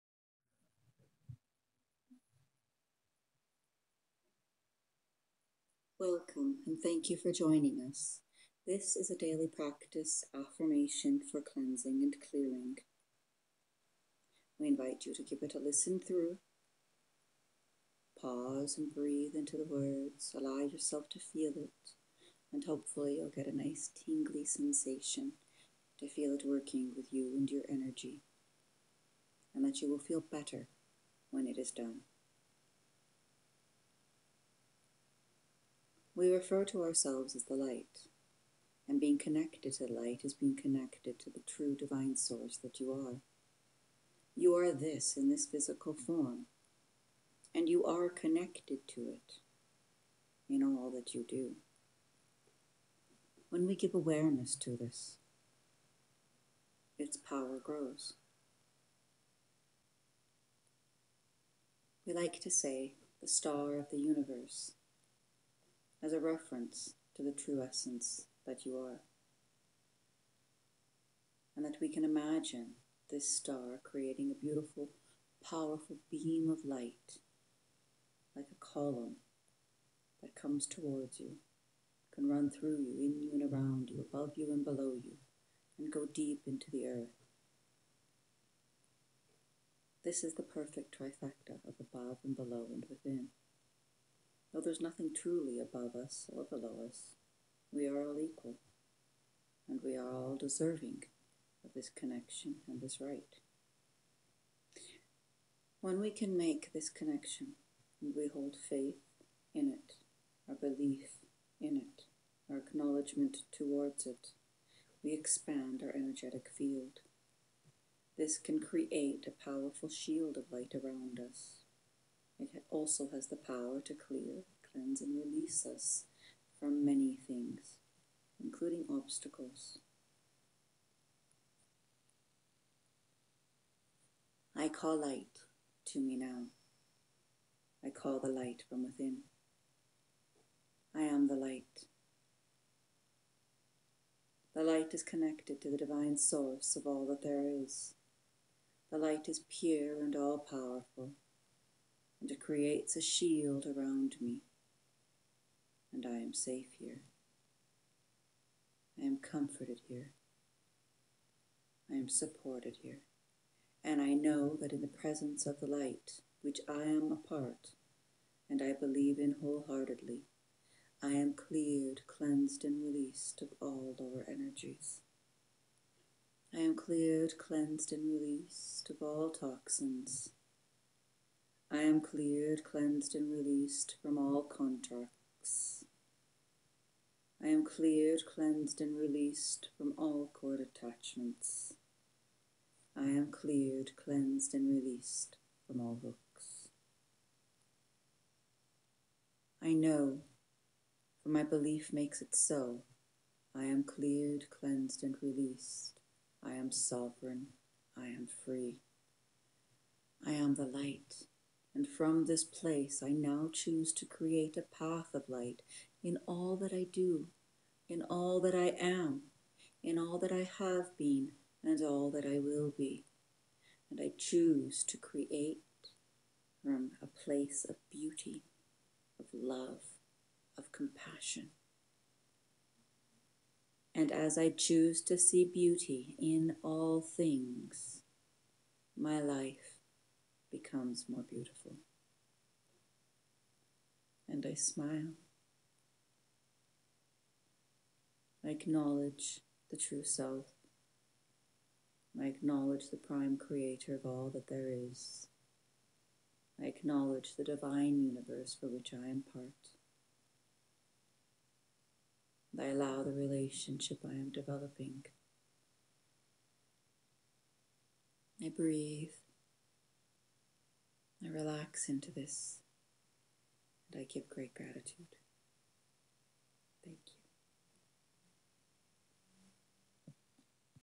5 minute Cleansing Light Meditation❤️
The sound is a bit low on this, please use headphones to listen :)
This is a fast and easy way to take care of you and your energy in a few minutes a day. A simple easy to affirm or invoke guided audio to clear and cleanse your energy and help you feel lighter and better.
Take your time to feel this, pause and breathe into the words & hopefully with the visualization, you should get a tingly effect - positive impact on energy.
(remember the true essence of you, the star of the universe - the true self, and the powerful column of light that is connected to you, going through you, in you, and around you, then deep into the Earth - when you connect with the higher self and the Earth is create a perfect trifecta of power which then can expand your energetic field and create a sphere of light around you, it has the power to clear cleanse and release you from obstacles, it has the power to raise your vibration, that has the power of connectivity, it has the power to help with your intentions, and whatever path you choose)
"I call light to me now.  I am the light, the light is within me, the light is connected to the Divine source of all that there is, the light is pure and all powerful and creates a shield around me, I am safe and comforted here, and I know that in the presence of the light, for which I am part, and I believe in wholeheartedly, I am cleared cleansed and release of all lower energies, I am cleared cleansed and released of all toxins, I am cleared cleanse and release from all contracts, I am cleared cleanse and release from all cord attachments, I am cleared cleansed and released from all hooks. I am Sovereign and I am free, I am the light. From this place I now choose to create a path of light and I choose to see beauty in all that I do, all that I am, and all that have been and all that I will be."
Always close by acknowledging the higher self and saying "thank you" - for this is the relationship you are developing.

Blessing of Light,
Cassaundra